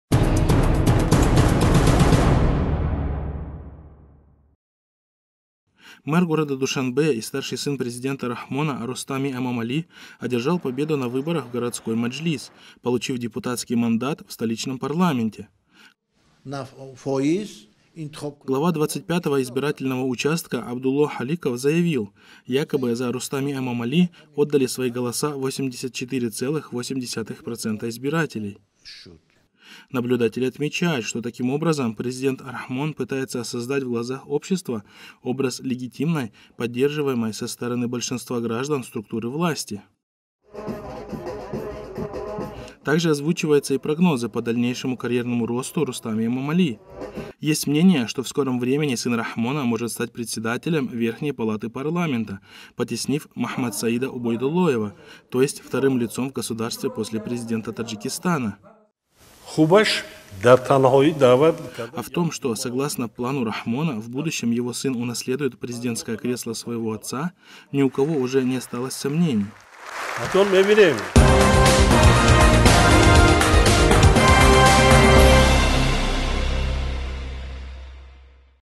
Невероятный карьерный рост Рустами Эмомали
Мэр города Душанбе и старший сын президента Рахмона Рустами Эмомали  одержал победу на выборах в городской маджлис, получив депутатский мандата в столичном парламенте. Глава 25-го избирательного участка Абдулло Холиков заявил, якобы за Рустами Эмомали отдали свои голоса 84,8% избирателей.